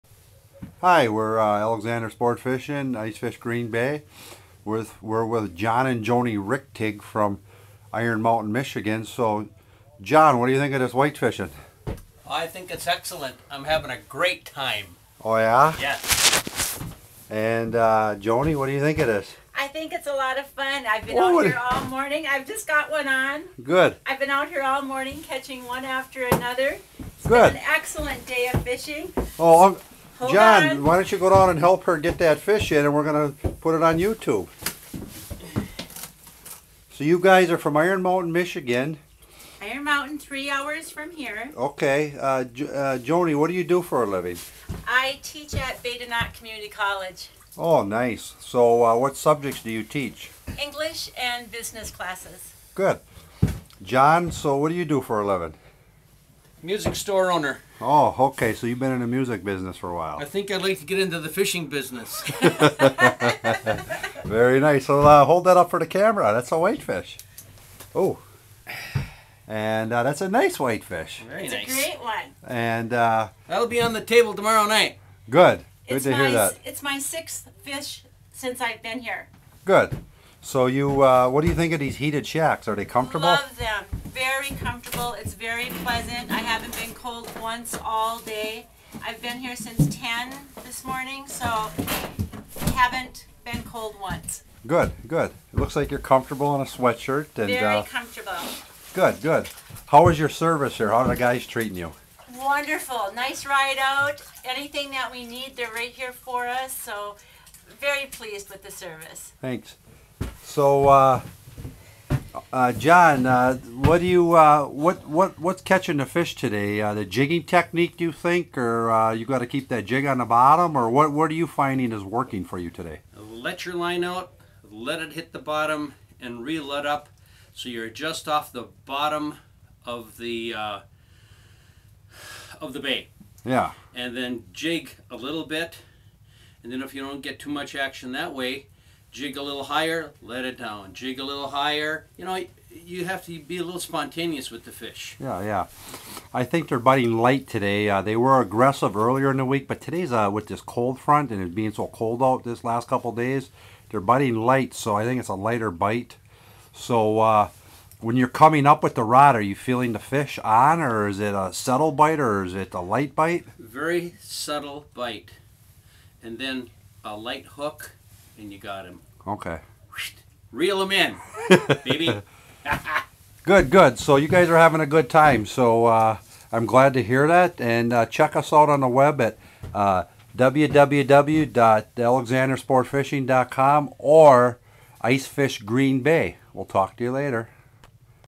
Michigan Clan catching Whitefish on Camera with Alexander Sportfishing
Great family from Iron Mountain Michigan catching whitefish limits